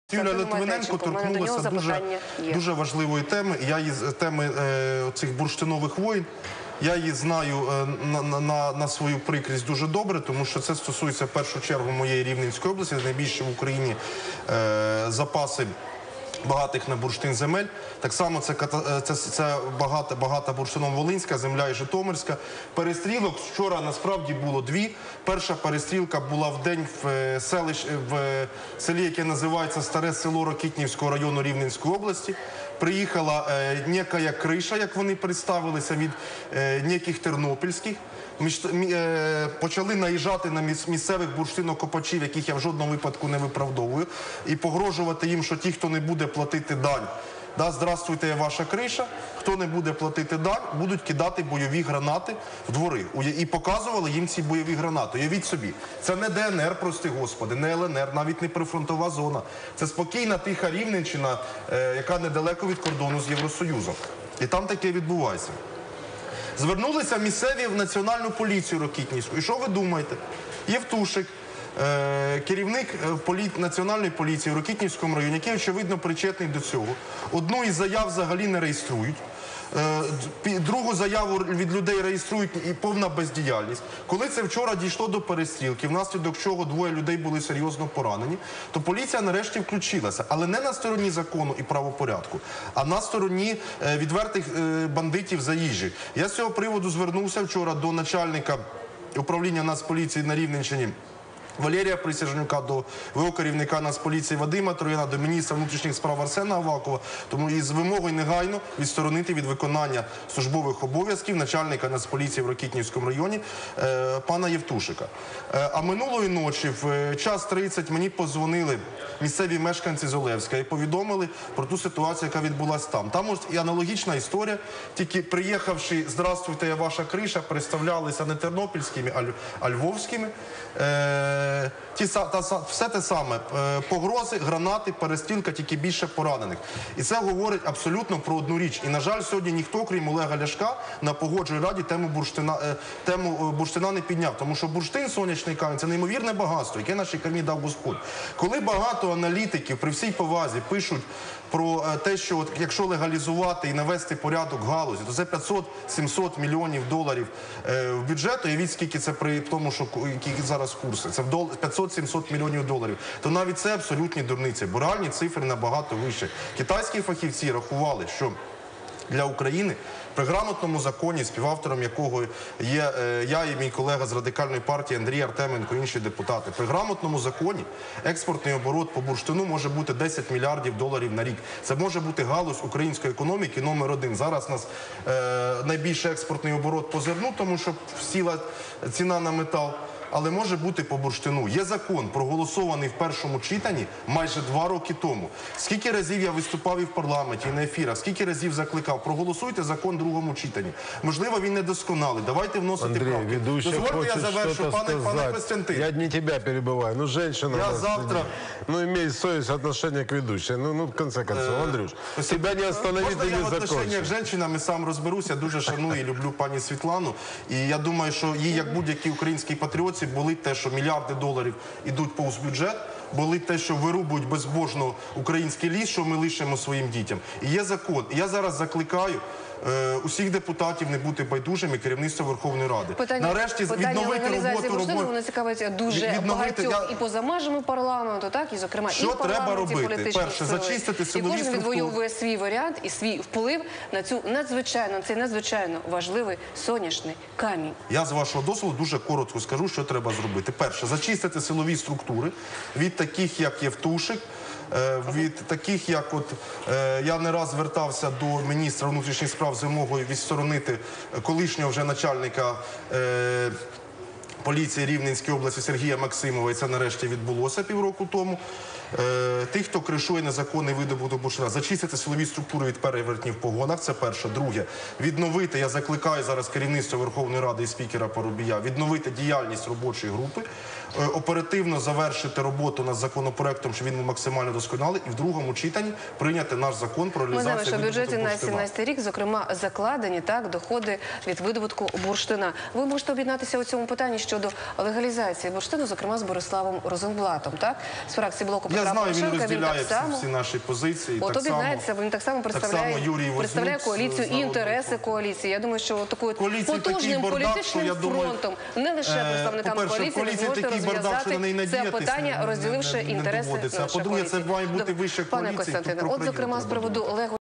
Лозовий: Легалізація бурштину може врятувати нашу економіку
Нардеп від Радикальної партії Андрій Лозовий розповів подробиці сутичок, які відбулися за останні декілька днів на Рівненщині між бурштинокопачами.
Політик наголосив, що очільник обласної поліції не реагує належним чином на звернення і відкрито підтримує одну із сторін конфлікту.
Нардеп також пояснив, які наразі перспективи легалізації видобутку бурштину та які наслідки матиме ухвалення цього закону для економіки країни.